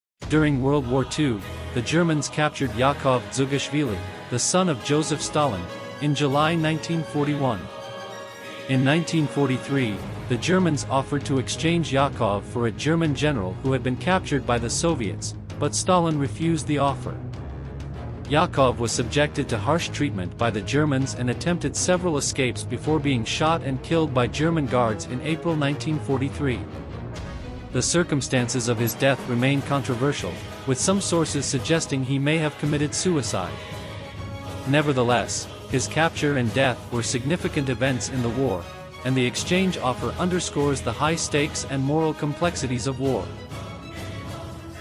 Execution Of Stalin's Son
The Execution Of Stalin's Son

During World War II, the Germans captured Yakov Dzhugashvili, the son of Joseph Stalin, in July 1941. In 1943, the Germans offered to exchange Yakov for a German general who had been captured by the Soviets, but Stalin refused the offer. Yakov was subjected to harsh treatment by the Germans and attempted several escapes before being shot and killed by German guards in April 1943. The circumstances of his death remain controversial, with some sources suggesting he may have committed suicide. Nevertheless, his capture and death were significant events in the war, and the exchange offer underscores the high stakes and moral complexities of war